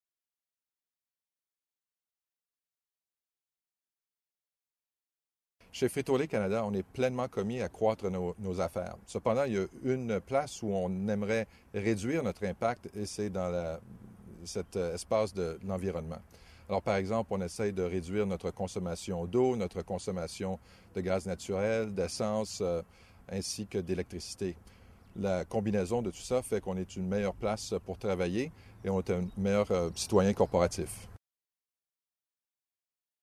Frito Lay Canada ajoute des véhicules électriques
Six véhicules électriques à émission zéro seront ajoutés à l'échelle du pays.